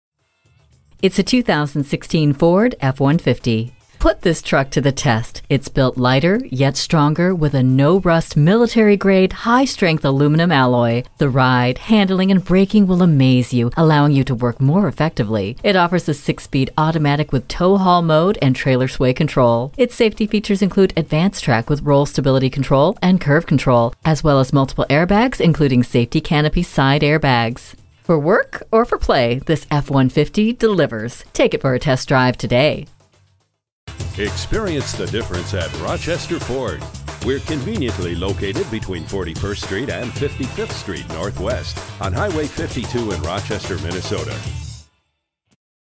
New 2016 Ford F-150 Rochester MN Winona, MN
Year: 2016
Make: Ford
Model: F-150
Engine: EcoBoost 3.5L V6 GTDi DOHC 24V Twin Turbocharged
Transmission: 6-Speed Automatic Electronic
Stock #: F168145
VIN: 1FTEW1EG8GKD44254

Address:
4900 Hwy 52 North